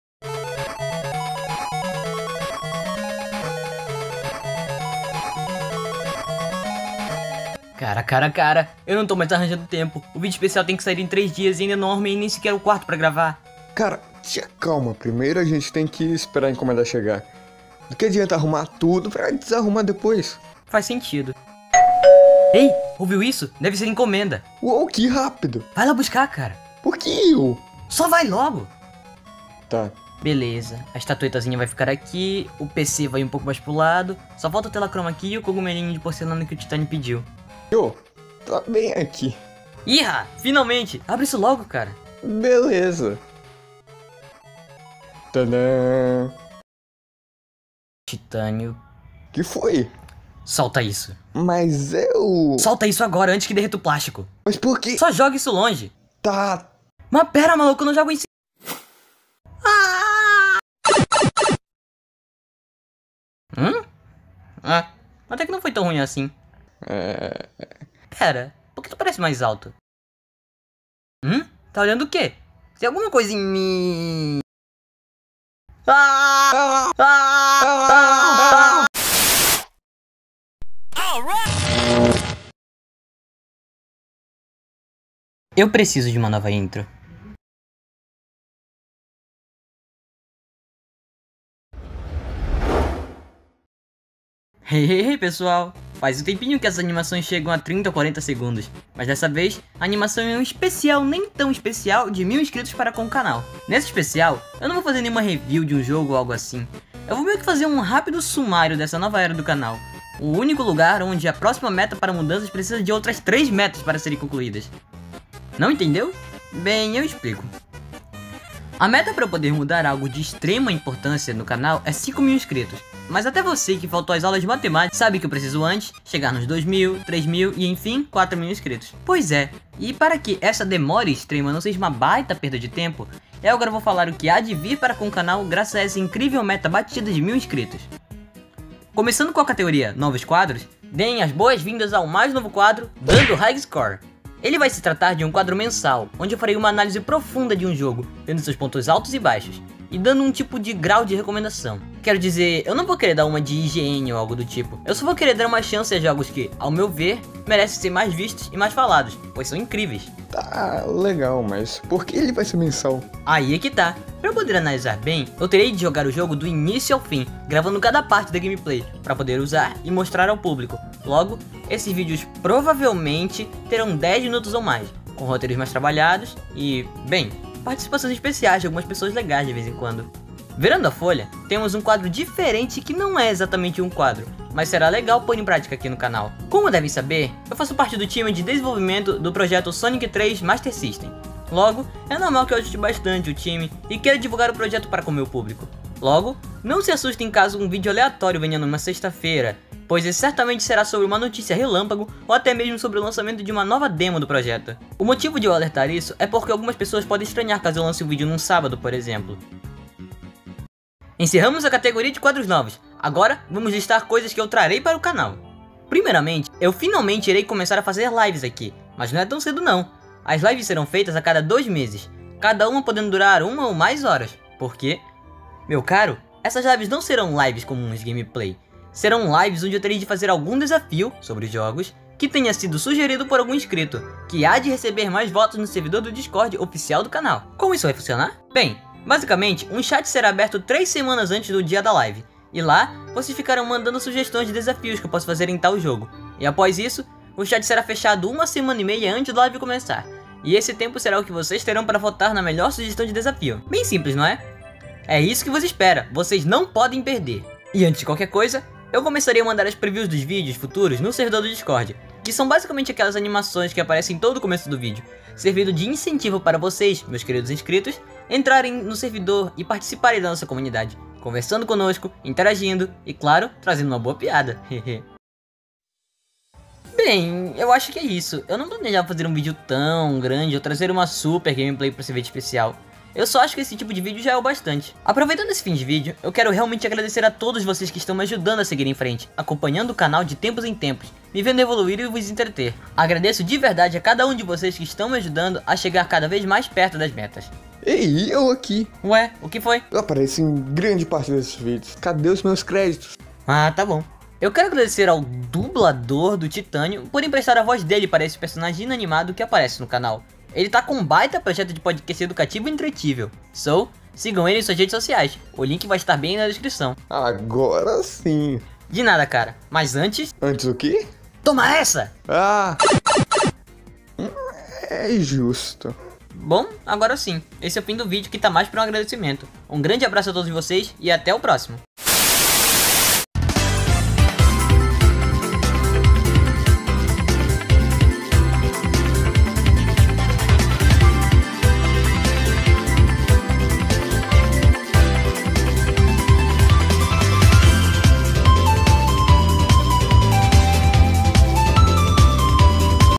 Especial nem-tão-especial de 1000 INSCRITOS! - Hidrogênio Falante
-Abra a descrição para ler a descrição-

Pois é, finalmente chegamos a incrível marca de 1000 inscritos no nosso querido canal!
Passamos por muitas coisas e delays para chegarmos até aqui, e acho que agora é hora de uma DRÁSTICA MUDANÇA.

Sim, Hidro não mudou de OC a toa, tudo isso tem um objetivo.

Quero agradecer a todos os que estão a me acompanhar até o momento, assistindo a meus vídeos, e sendo entretido pelos mesmos, isso me dá uma baita razão para continuar a fazer ótimos vídeos!

|Links|
Link do servidor do Discord:

-Estamos impulsionados na velocidade da luz!-